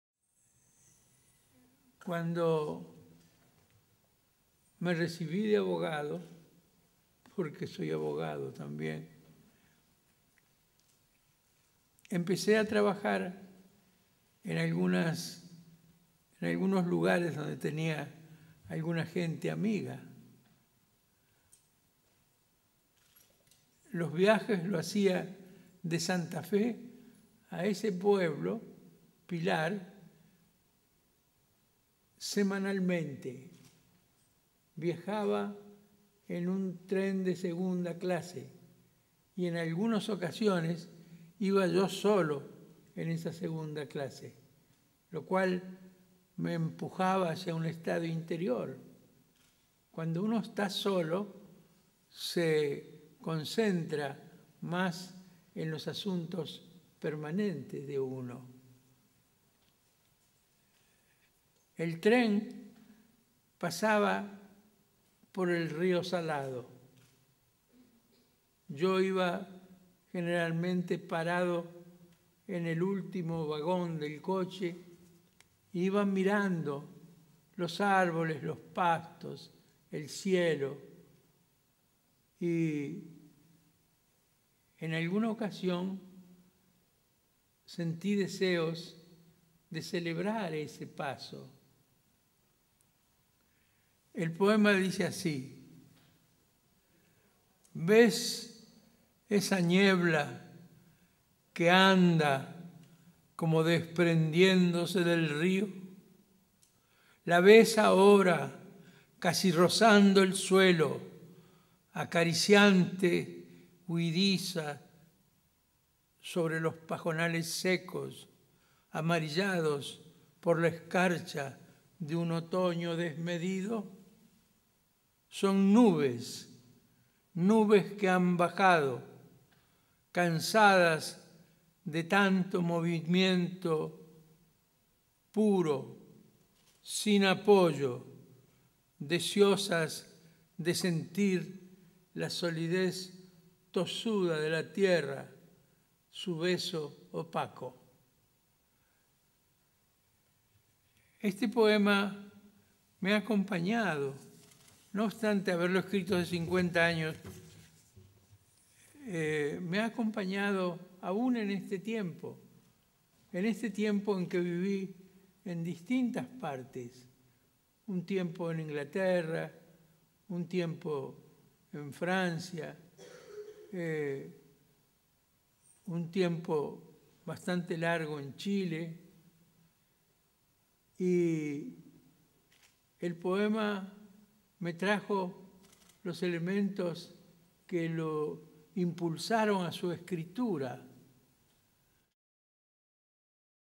Hugo Gola en el XIX Festival Internacional de Poesía de Rosario
Lectura del distinguido poeta santafesino Hugo Gola en el XIX Festival Internacional de Poesía de Rosario / Teatro Príncipe de Asturias, Centro Cultural Parque de España (Rosario, Argentina)  / Septiembre 2011